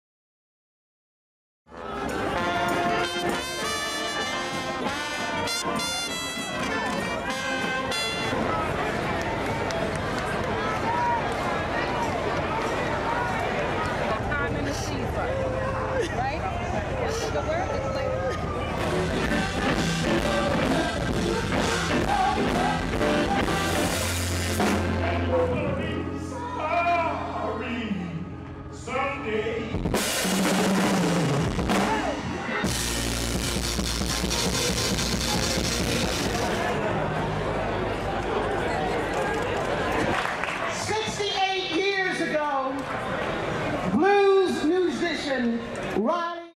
Memphis pays final tribute to blues legend B.B. King
The city where B.B. King got his musical start paid final tribute to the blues guitar master Wednesday, as thousands walked - and played - alongside his hearse